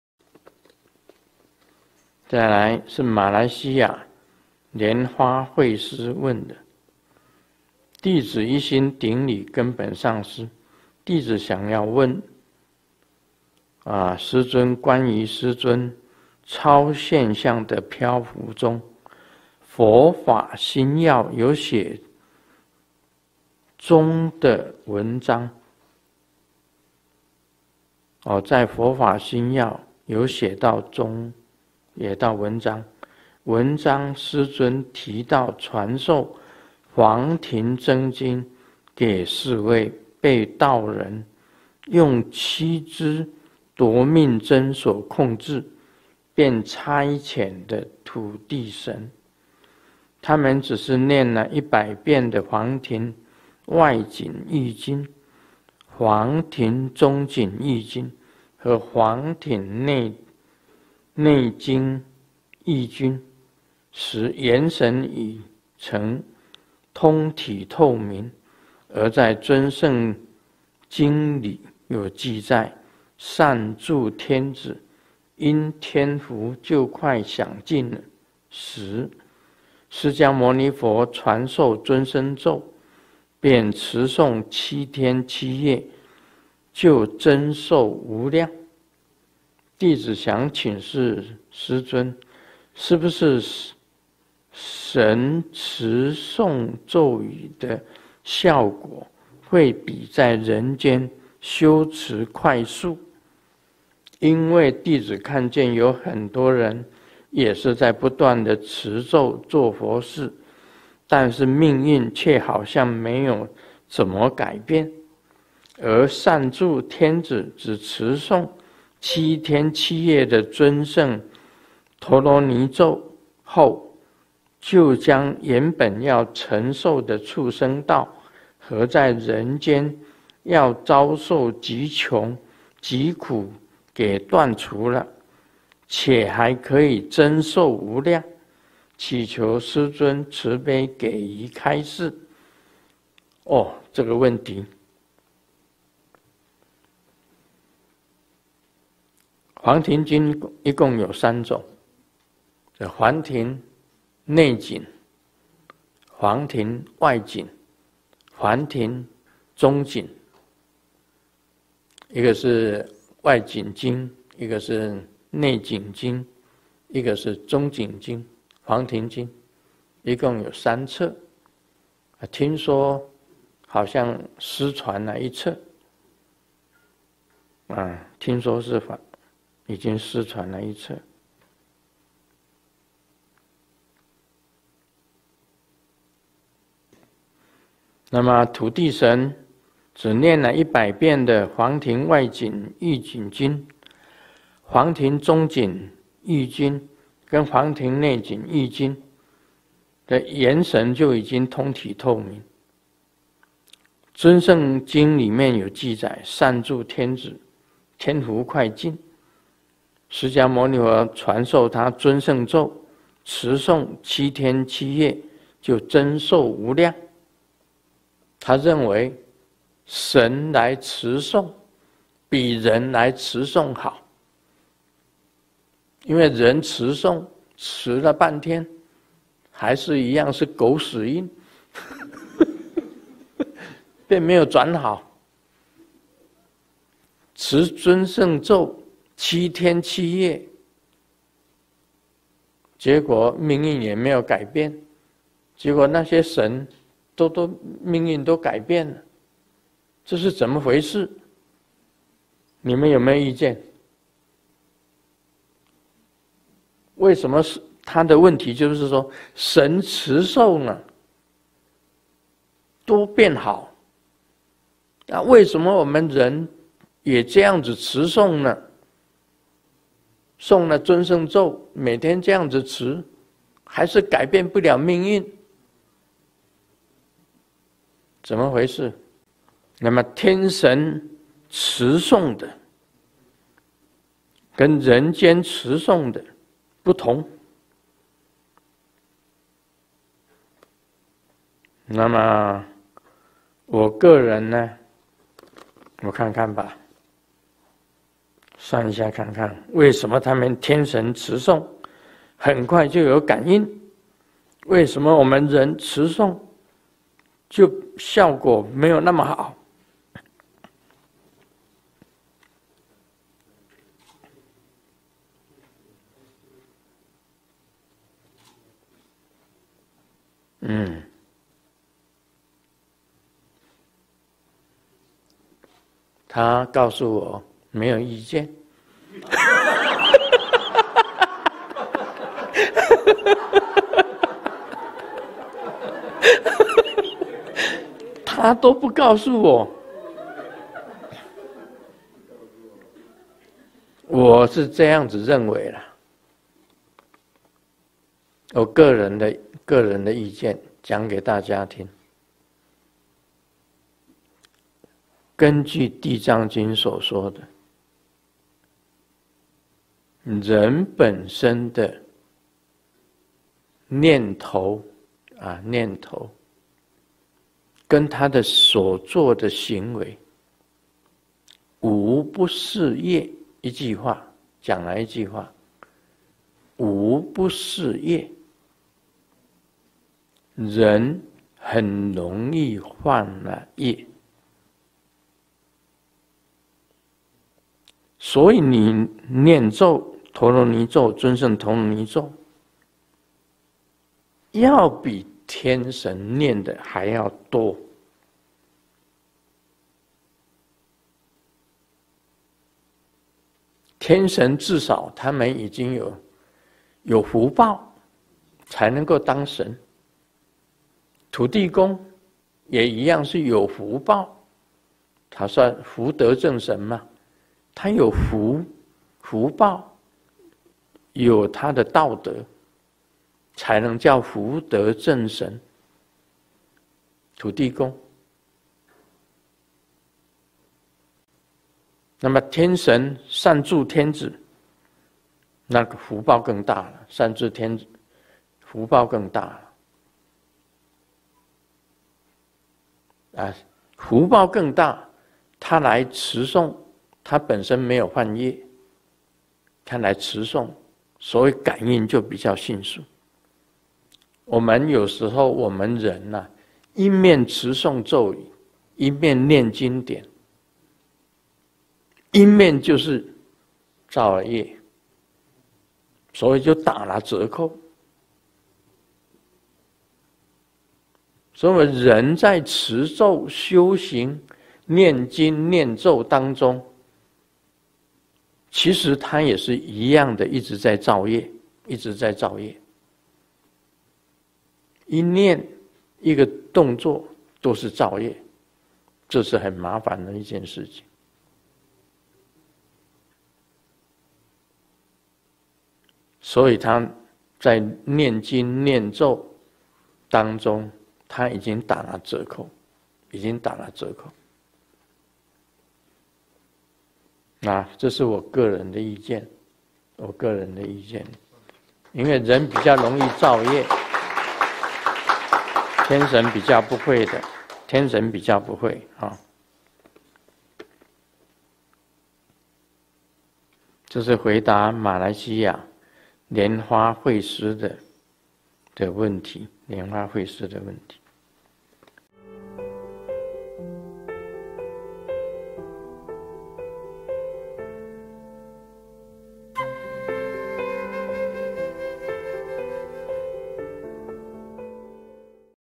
25-04-2021 Perbedaan Hasil Bhavana Antara Manusia dan Dewa 人很容易犯了業，所以唸經咒要比神明唸的要多
Tahukah Anda mengapa Dewa menjapa mantra lebih cepat membuahkan hasil daripada manusia? Penasaran? Mari kita dengarkan jawabannya di dalam cuplikan video berikut ini.

20210425聖尊蓮生活佛盧勝彥解答疑難4：人很容易犯了業，所以唸經咒要比神明唸的要多－TBSN高清版
Rainbow Temple